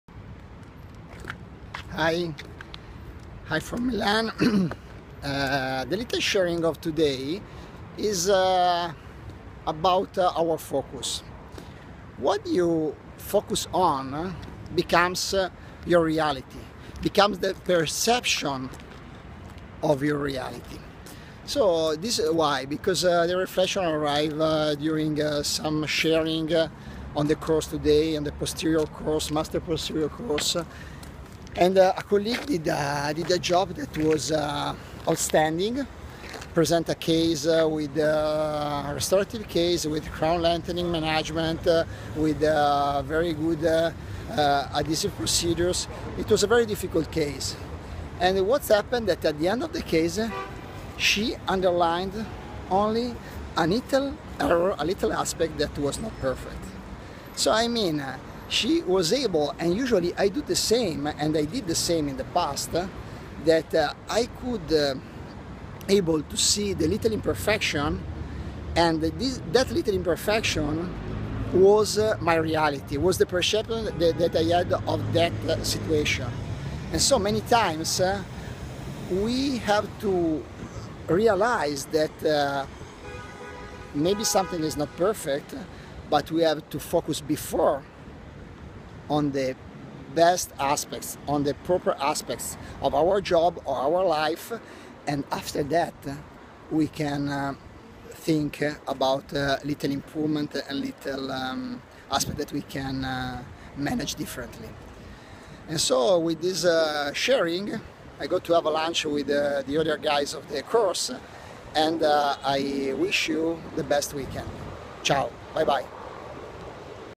What you focus on becomes your reality VISION & ACTION 12 by Federico Ferraris
Focus before on the positive aspects and then think what and how is possible to improve! If you start to focus on negative aspects, your reality will be negative, or better you will perceive a negative reality! Start to focus with the beauty of your daily life! 09 May 2015